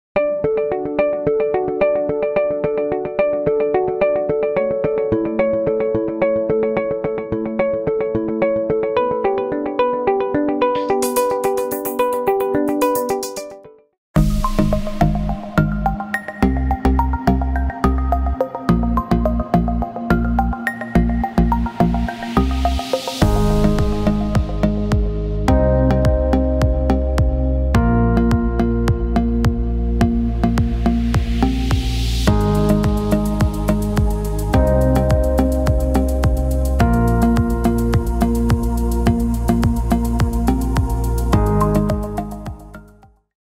Green screen , After Effects production 🎙 ,FREE TO USE.
Add your photos and videos after removing the green space to view your report. You can also add the text you want to add. Channel subscribers can use this material for free after modifying it.